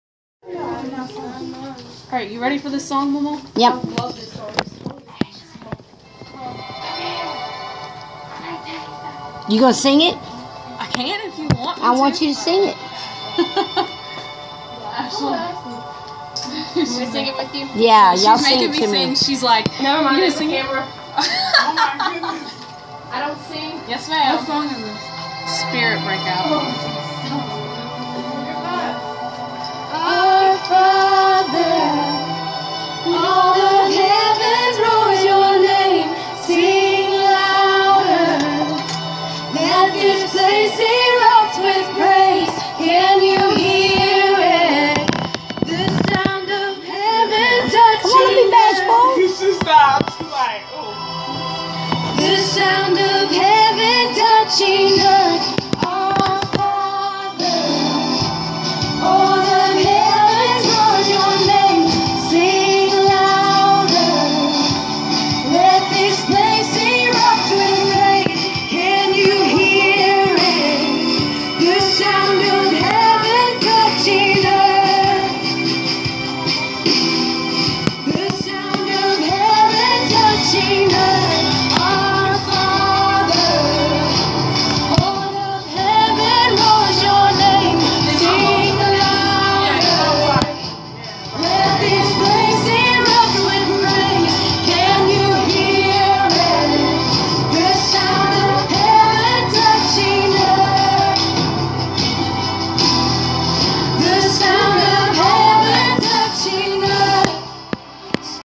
Jam session at Mary's Heart Cathilic Gift Store...
My beautiful grandaughter Victoria Ducote singing for me at the store